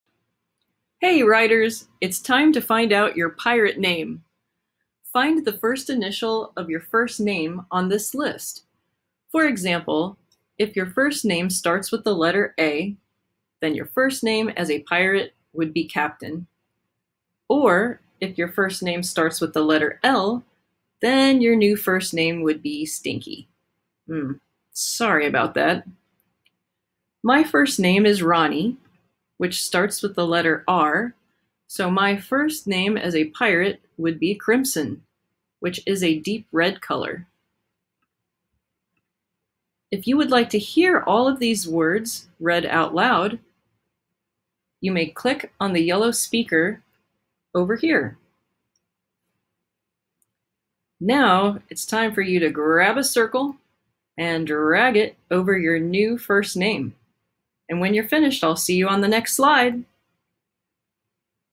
What Is Your Pirate First Name? Find out in this Google Slides lesson! Kick off pirate writing fun!
This video is the first part of a warm-up activity for my Pirate Adventure-themed writing class. Kids discover their first name as a pirate...and also end up with a helpful "word bank" of descriptive words that they may use later in the workshop while brainstorming pirate characters, ship names, settings on treasure maps...and more! Parents and teachers may also use the resource as a fun warm-up for their own pirate-themed activities! Happy swashbuckling, and happy writing!